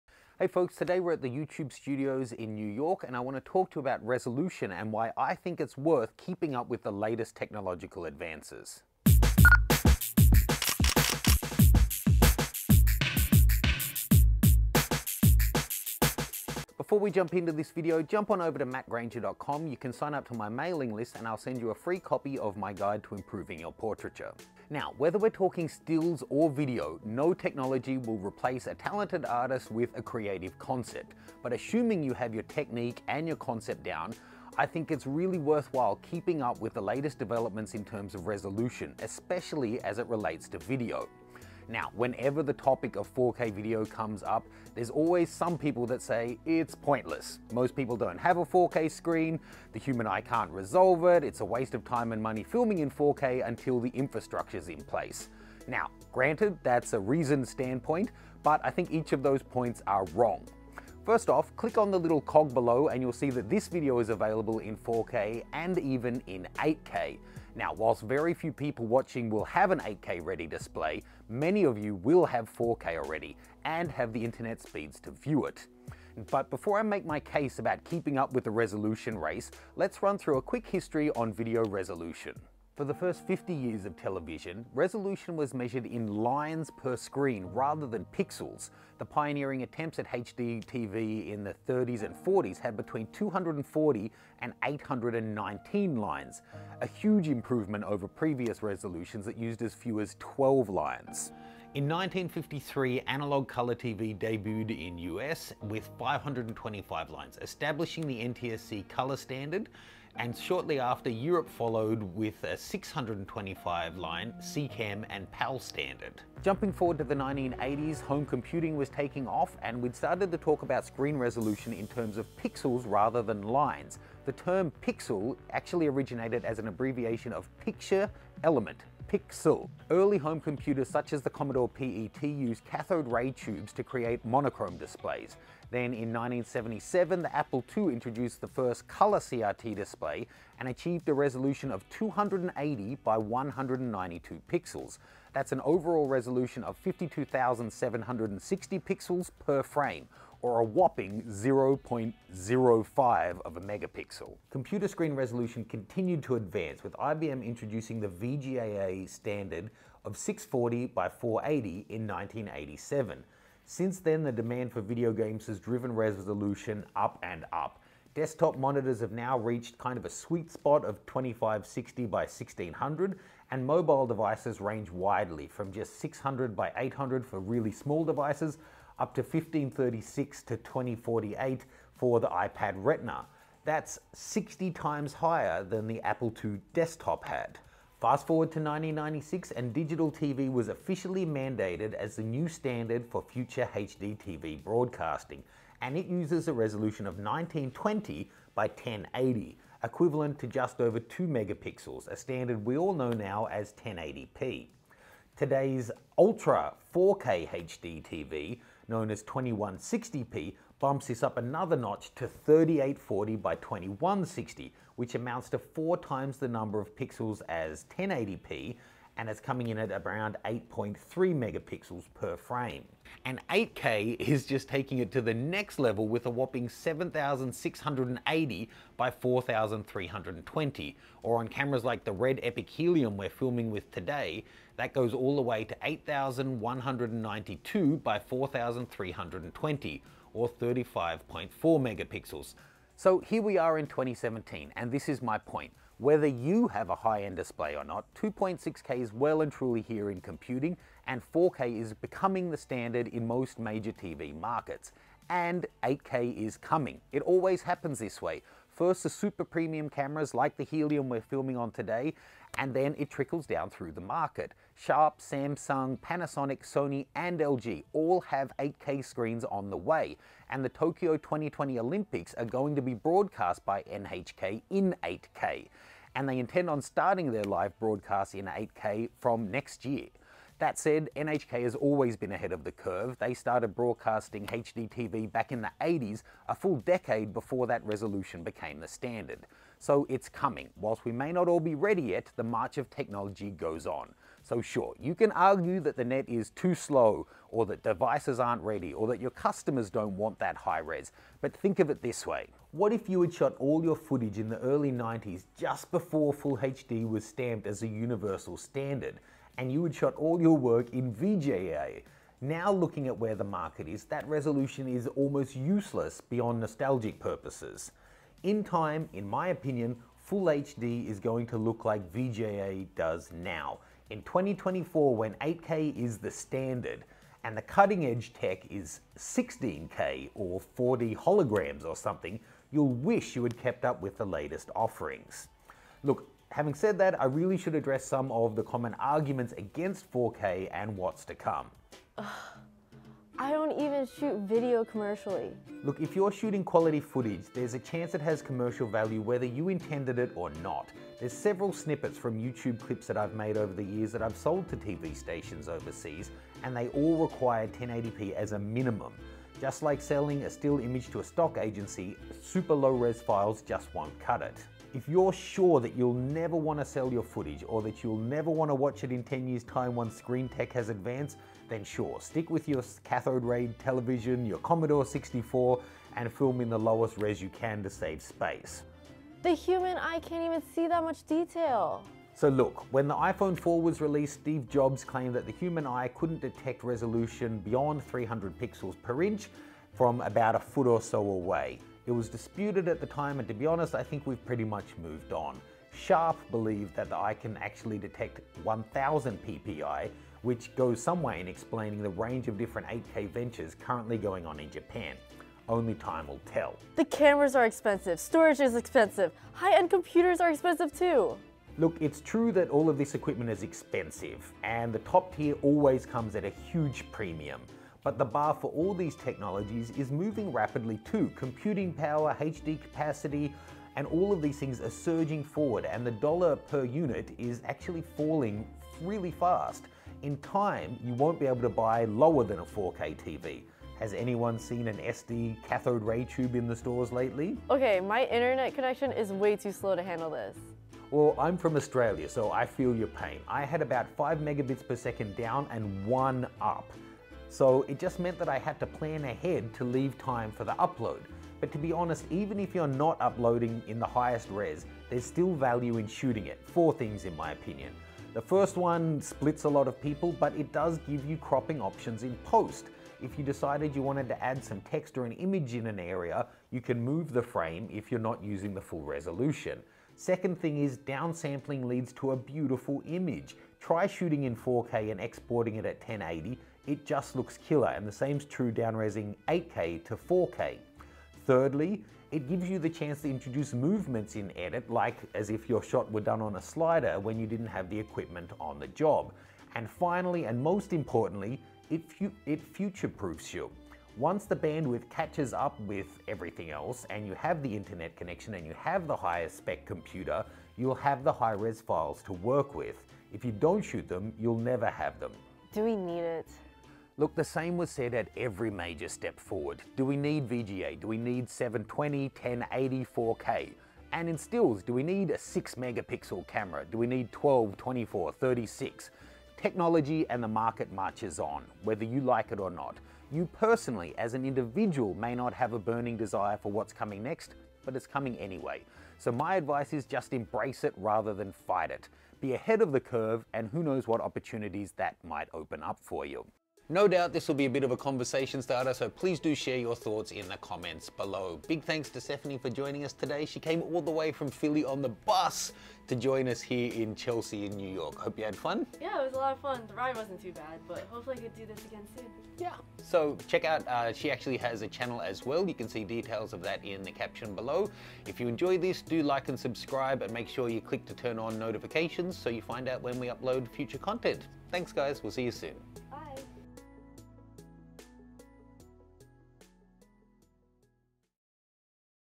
8K - The Future of Video Resolution
8K is cutting edge at this stage - and to be honest - editing this video was a strain. We filmed two short videos in 8k, with lots of 4k b-roll and ended up with almost a terabyte of data! Add to that, the RED Mags are reallllly slow to transfer off - so I had to go back the next day to continue dumping the footage.

The camera is the highest end element in our workflow - our computer, monitor and storage wasn't up to the task. They all crush 4K - but 8K was too much. Give it a couple of years and it will all have caught up - and by then 12k sensors will be on the market.

That said - 4K is here. We are filming and uploading 4k regularly now. And once 6K and 8K drop into the upper pro-sumer range, we aim to be early adopters.